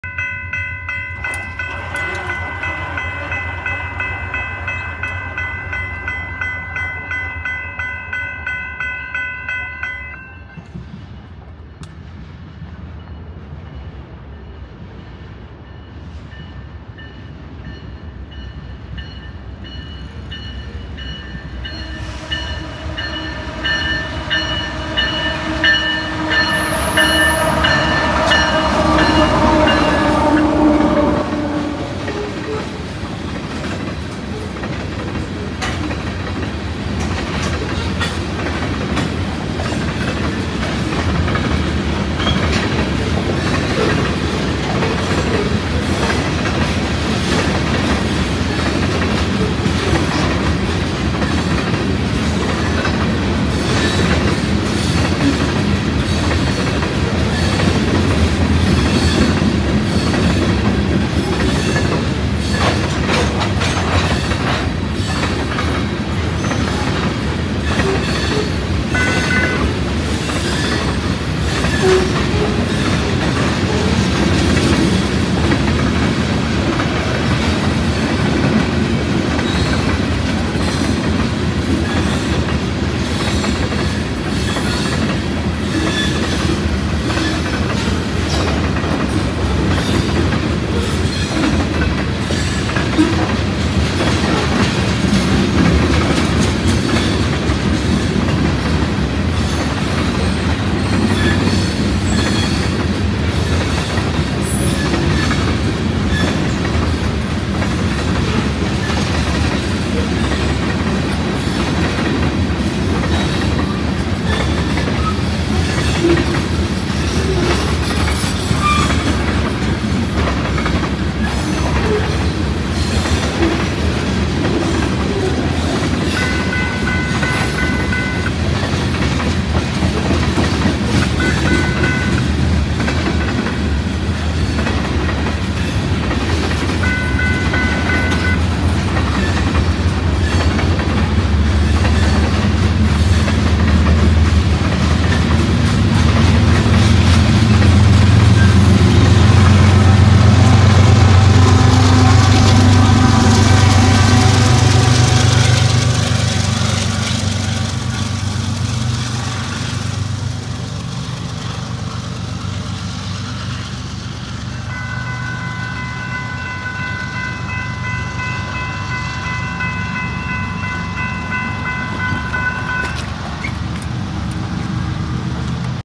CP Grain Train In LaGrange, IL On November 3, 2024
I caught a CP Grain Train with CP 8568 up front and KCS 4688 on the rear at 9:22 PM.